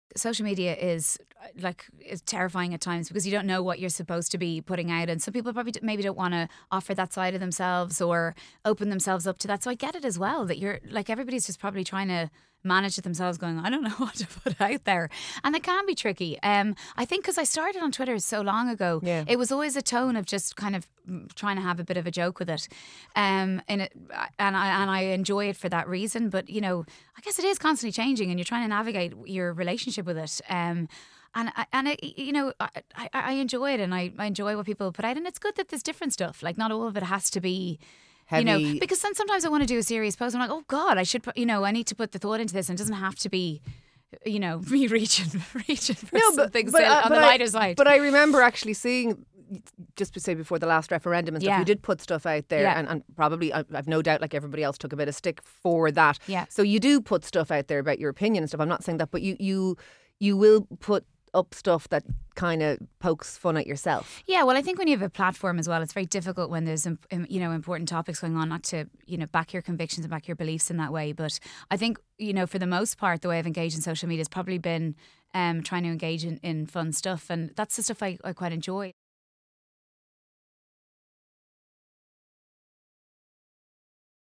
Amy Huberman On Navigating Social Media
Amy Huberman shares her thoughts on social media with Ciara Kelly.